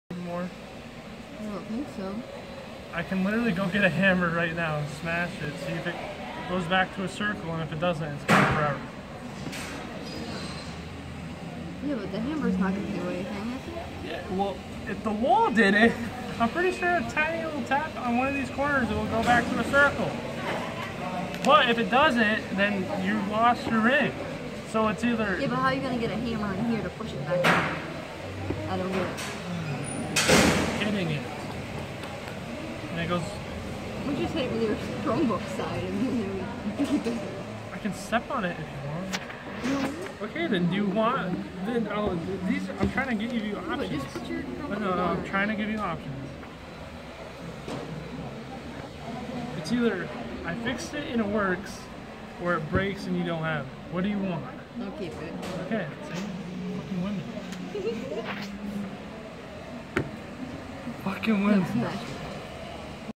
Just some random stuff during studyhall Pt.2
She bent her ring by attempting to slap me and instead hit the wall.